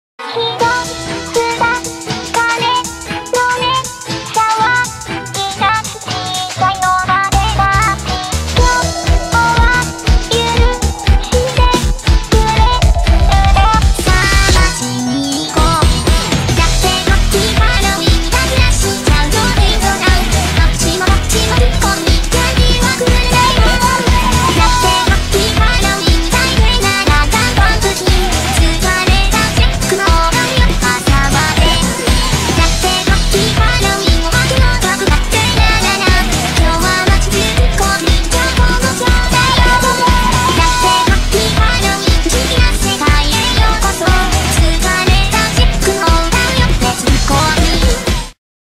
Happy Halloween | meme
Characters:
Tea, Hoshi, Cat, Lily, Kuro, Zig, Kutsuki
I want to apologize for a few things at once. Firstly, the videos did not come out for quite a long time, but there is an excuse for this — 9th grade and preparation for the exam. Secondly, I did not try very hard on the video, because in our country, that is, Russia, Halloween is not celebrated, so it didn’t bother too much
🐾Skype-Cookie_Cat_Waffle
Collabs-open🐱
Skype-open🐈